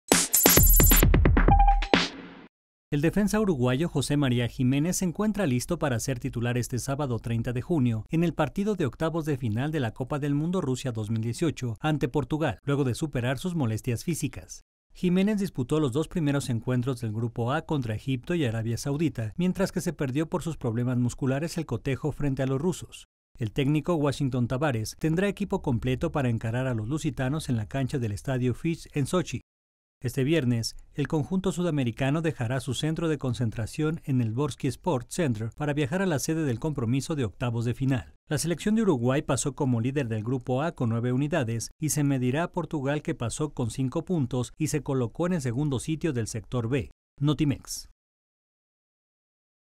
Uruguayo José María Giménez apunta a titularidad contra Portugal
El defensa uruguayo José María Giménez se encuentra listo para ser titular este sábado en el partido de octavos de final de la Copa del Mundo Rusia 2018 ante Portugal, luego de superar sus molestias físicas.